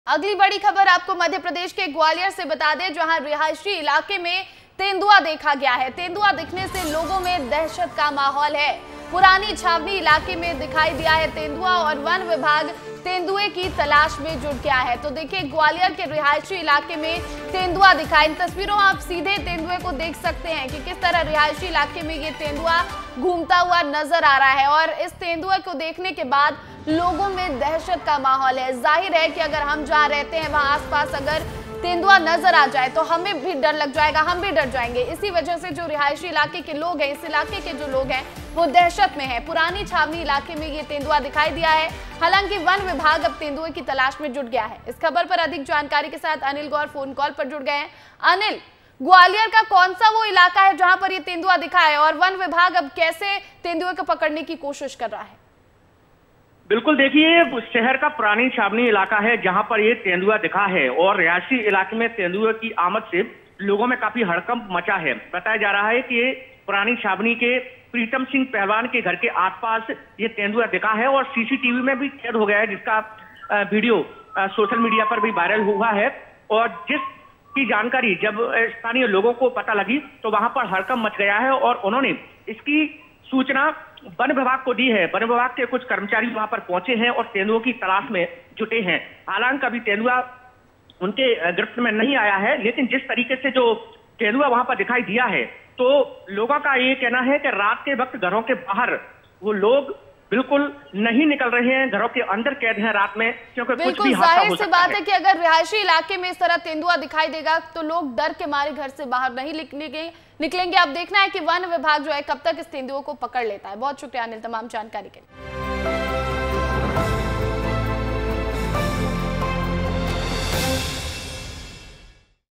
CG News : रिहायशी इलाके में घुसा तेंदुआ, VIDEO:बस स्टैंड के पास दहशत का माहौल
Breaking News : रिहायशी इलाके में तेंदुआ दिखने लोगों में दहशत, वन विभाग की तलाश जारी

we report on a recent sighting of a leopard in a residential area, causing widespread panic among local residents. The forest department is actively searching for the animal to ensure the safety of the community.